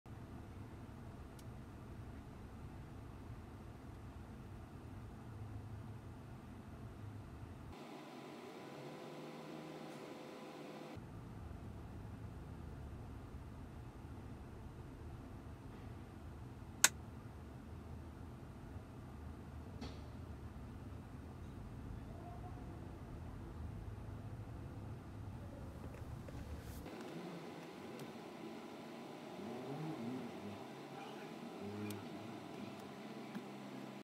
車CAM直擊 - 三個身位，照俾開門L 用袋同門整花架車
RA256 Benz 興發街
網友提供